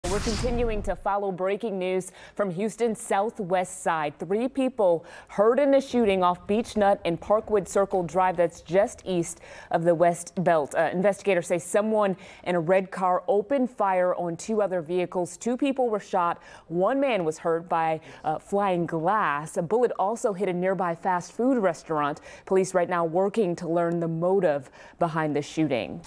3 people injured after suspect fires shots at vehicles in southwest Houston
Three people were injured after a suspect fired shots at two vehicles in southwest Houston Thursday, the Houston Police Department said.